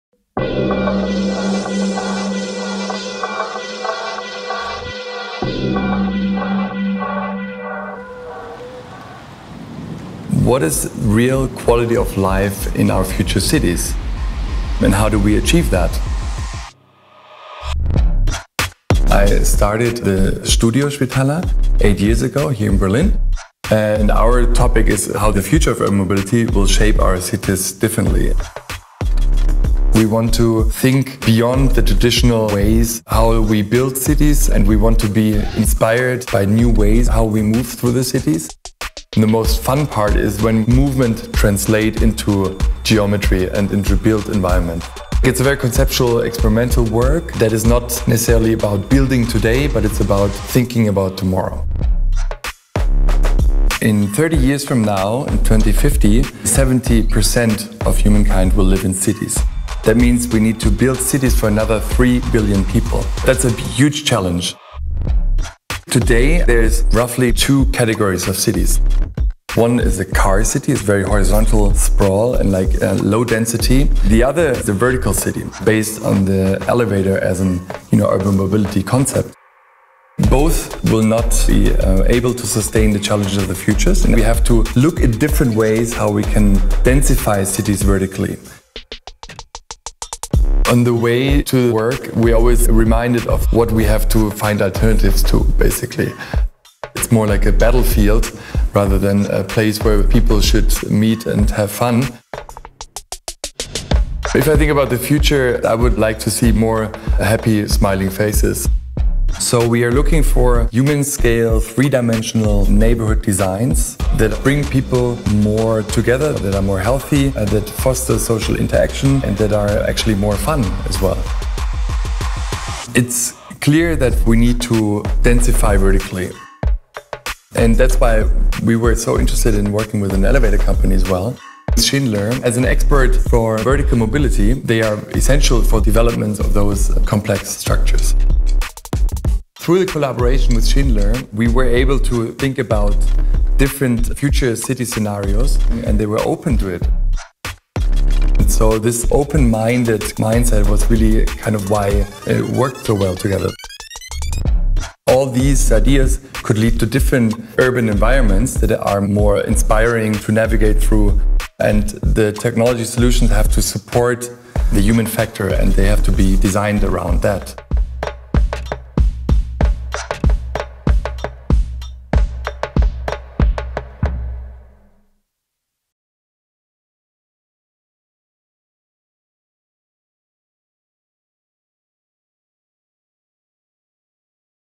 EXTRA VISIONARY
Wie sieht Lebensqualität in der Stadt der Zukunft aus? Und wie erreichen wir diese? Der Berliner Architekt Max Schwitalla hat sich die Suche nach Antworten auf diese Frage zum Beruf gemacht.